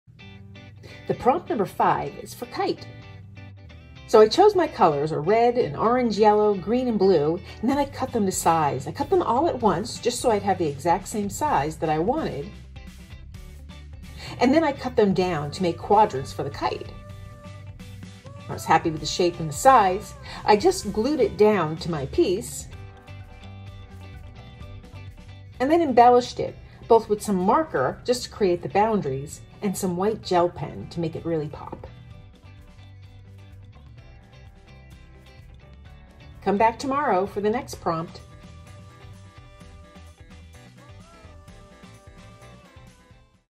10 Mini Summer Collages, Prompt 5: Kite
Join me for daily Collage Shorts Videos Monday, August 1 - August 10th. Each day, I'll post a short and sweet collage, showing the process of creating 10 mini collages, inspired by summer.
Each collage with be a simple image, made with colorful painted papers.
You can use  paper, washi tape, gel prints, scrapbook paper, or anything you can glue down.

10 Daily Prompts
August 1- Swimsuits
August 2- Flip Flops
August 3- Watermelon
August 4- Summer Hat
August 5 - Kite
August 6- Beach Ball
August 7- Ice Pop
August 8- Sunglasses
August 9- Snorkel
August 10- Surfboard